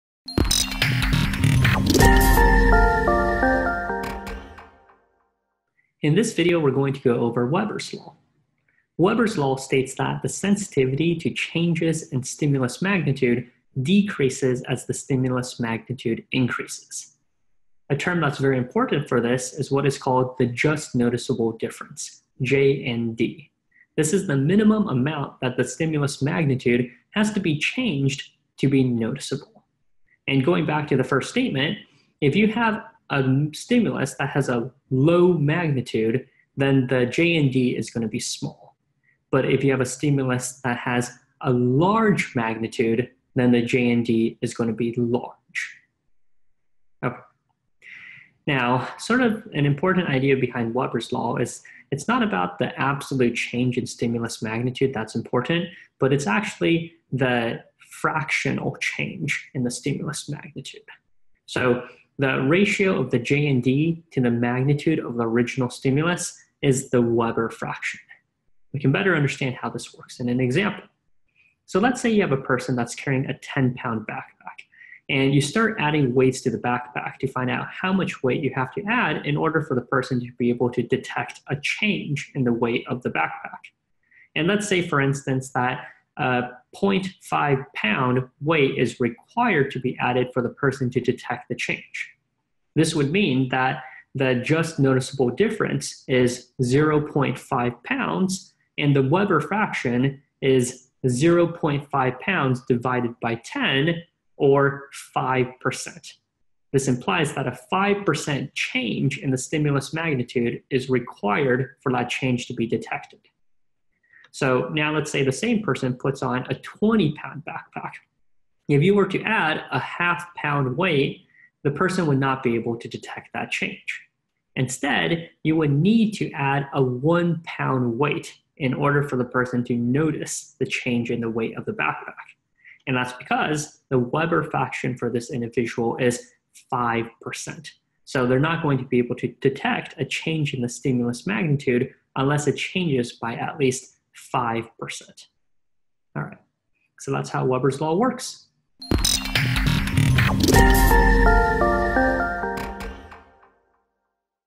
Weber's Law for Sensation
Need help preparing for the Psych/Soc section of the MCAT? MedSchoolCoach expert, Ken Tao, will teach everything you need to know about Weber's Law for sensation. Watch this video to get all the MCAT study tips you need to do well on the psychology section of the exam!

If the light in a room is extremely dim, we’ll probably notice if it gets a little brighter or dimmer. However, if a light is extremely bright, we probably won’t notice if the light becomes slightly brighter or dimmer. Weber's Law states that our sensitivity to a change in a stimulus decreases as the magnitude of the stimulus increases. A term that's very important for this is what is called the just noticeable difference (JND). The JND is the minimum amount that the magnitude of a stimulus has to be changed to be noticeable. Returning to our initial example, if the light is dim (low magnitude), its JND is low. If the light is bright (high magnitude), its JND is high.

JND can be quantitatively understood as the fraction of change in the original stimulus magnitude.

The ratio of the JND to the magnitude of the original stimulus is the Weber fraction, which is a constant that describes the percent change necessary to detect a change in stimulus at all magnitudes of the stimulus. For example, if you have a 10 lb backpack, and you require a 0.5 lb weight to detect a change in weight, there is a 5% change in stimulus magnitude required for that change to be detected. In other words, the Weber fraction is 0.5/10, or 5%. Therefore, if we had a backpack of any weight, it would need to increase or decrease in weight by 5% before we noticed the change. For example, we would predict a 30 lb backpack to require a 1.5 lb increase or decrease in weight to notice a change.

So let's say you have a person that's carrying a 10lb backpack, and you start adding weights to the backpack to find out how much weight you have to add in order for the person to be able to detect a change in the weight of the backpack. And let's say for instance, that a .5lb weight is required to be added for the person to detect the change. This would mean that the JND is 0.5lbs, and the Weber faction is 0.5lbs divided by 10 or 5%. This implies that a 5% change in the stimulus magnitude is required for that change to be detected.

MEDSCHOOLCOACH